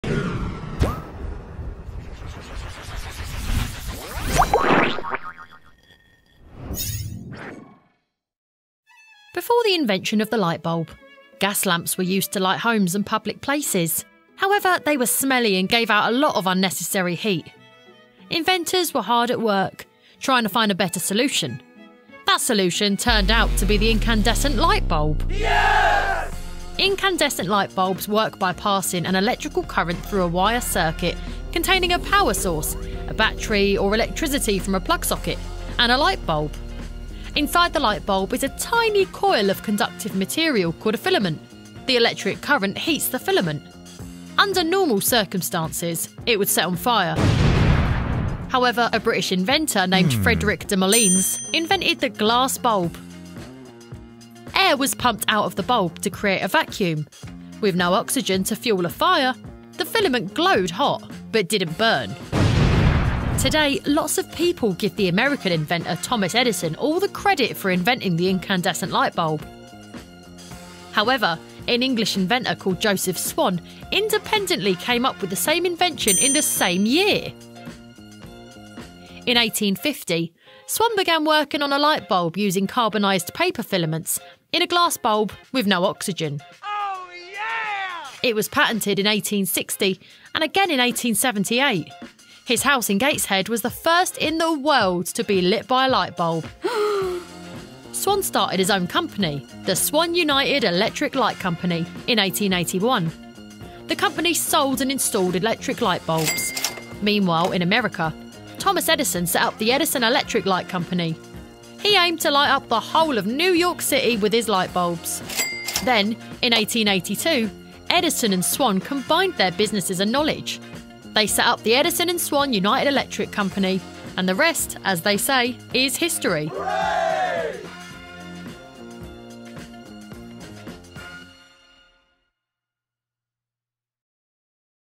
The Invention of the Light Bulb
A little history of the creation and development of the lightbulb. Starting with the fastest person first and ending up with a very successful rivalry.